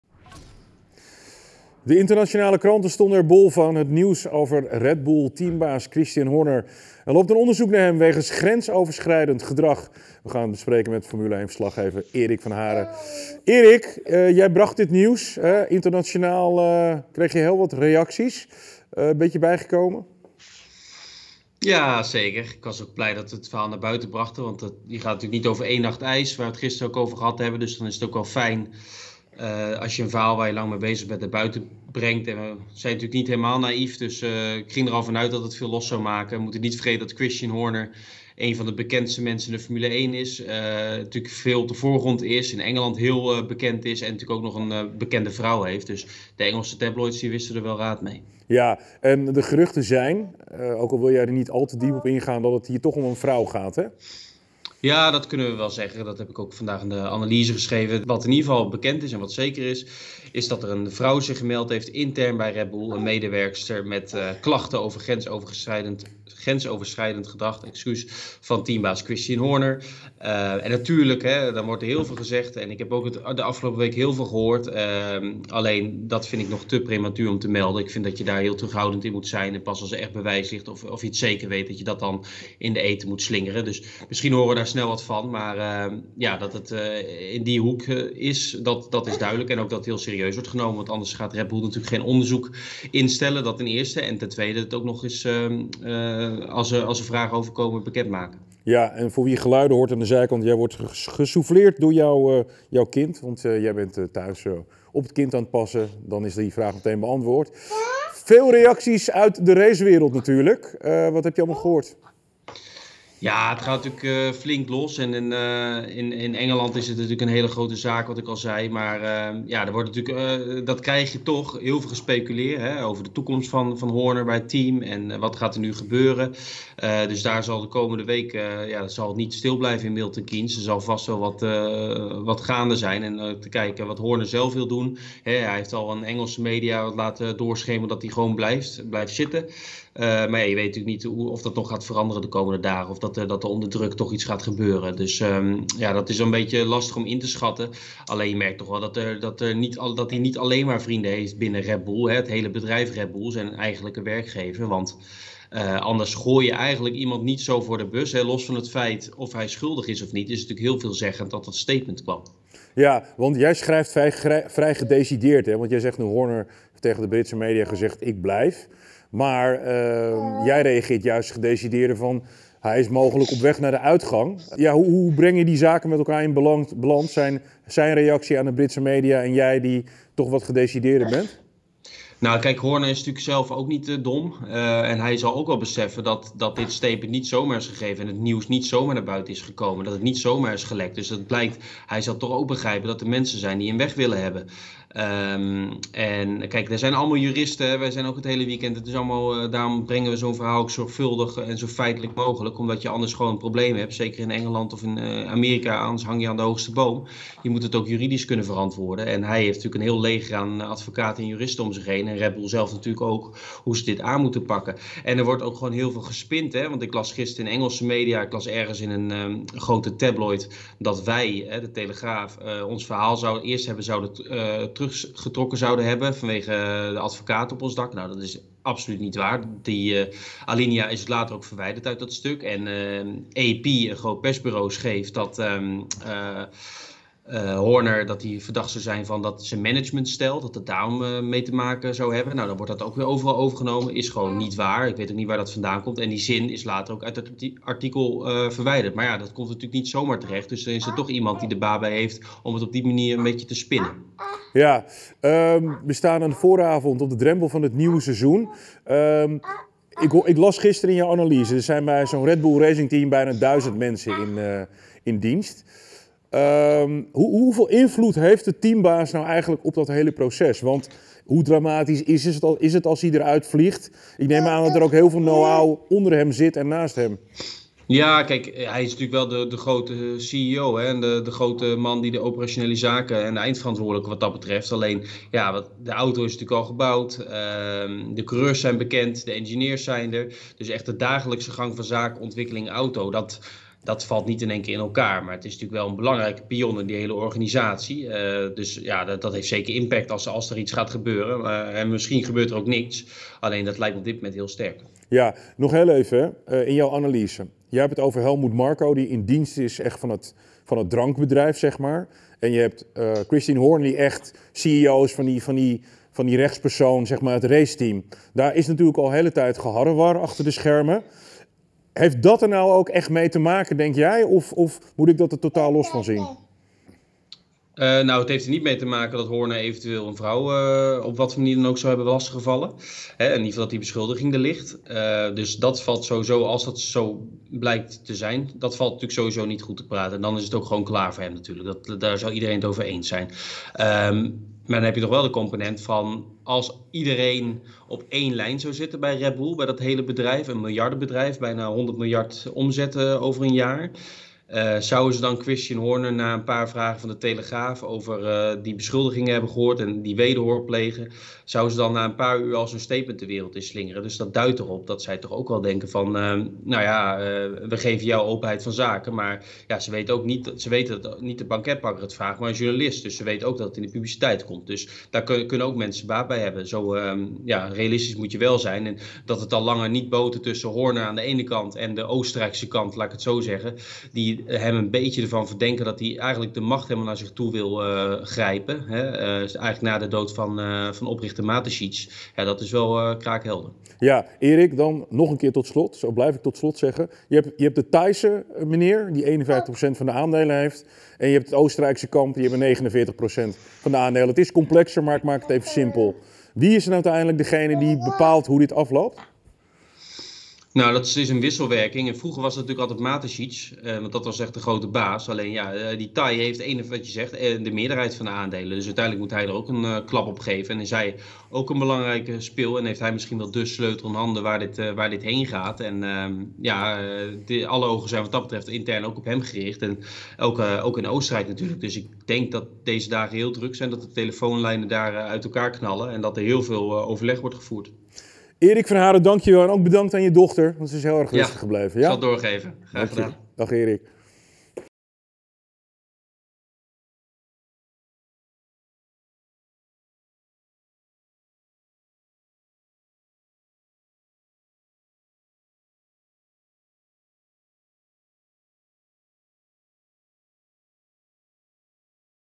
'Hij bepaalt waar dit heen gaat bij Red Bull'
De internationale kranten stonden er bol van: het nieuws over Red Bull-teambaas Christian Horner. Er loopt een onderzoek naar hem wegens grensoverschrijdend gedrag. Het nieuws kwam van Formule 1-verslaggever Erik van Haren, die ziet dat het bij Red Bull heel serieus wordt genomen.